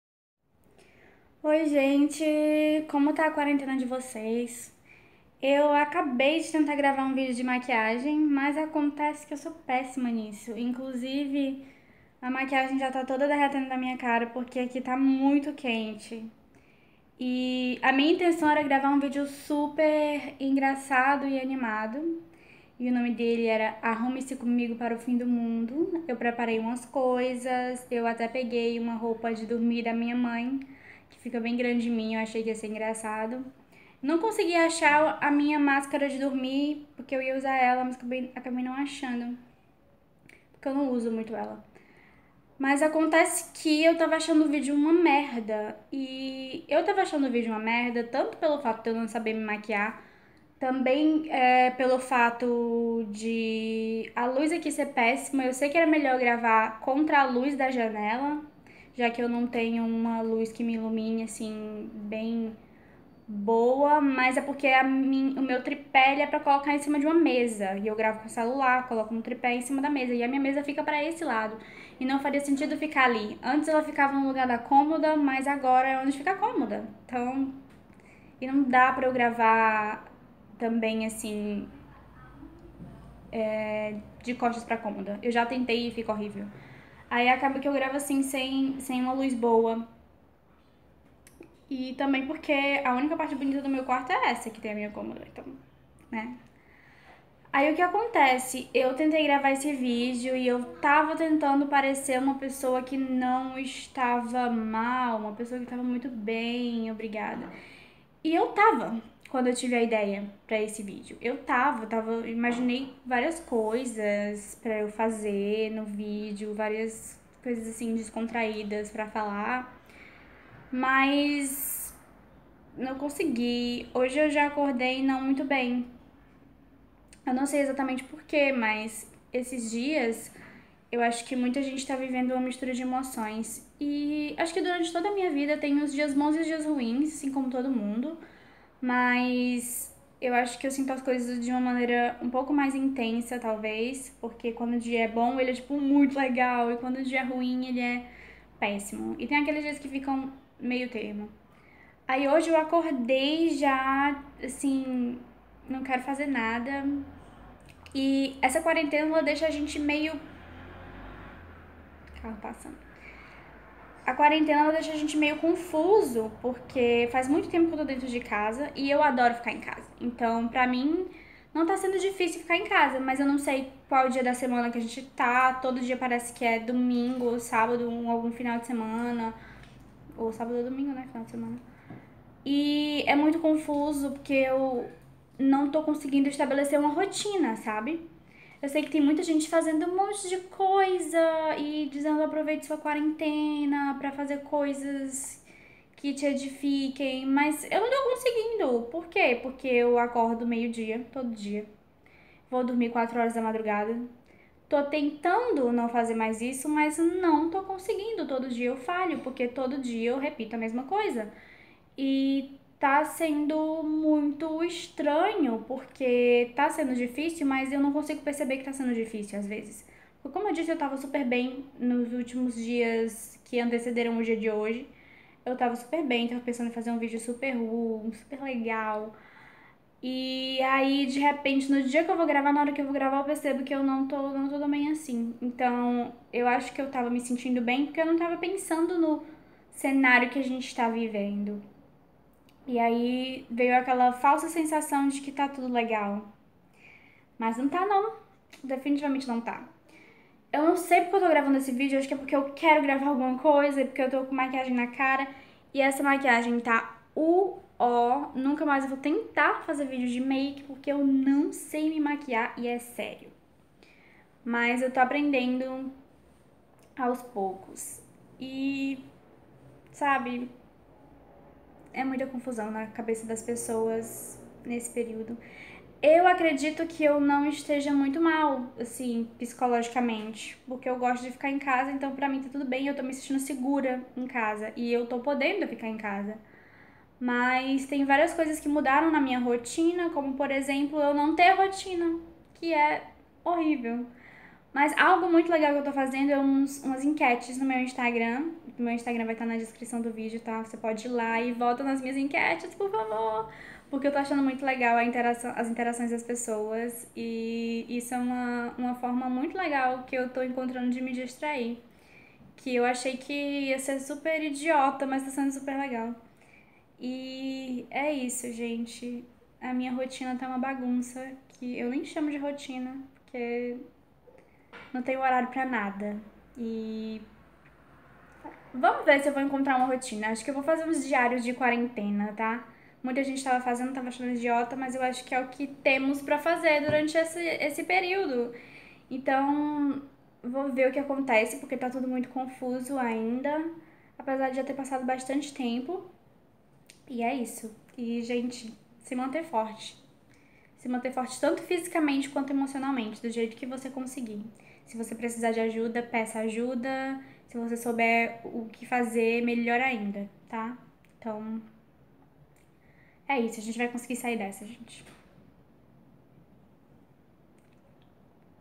NÃO CONSIGO MANTER UMA ROTINA NA QUARENTENA | Diário de Quarentena?
Esse é um momento muito difícil para todos nós e precisamos entender que não saber lidar com ele é super normal.

twitter e instagram: @danycartwright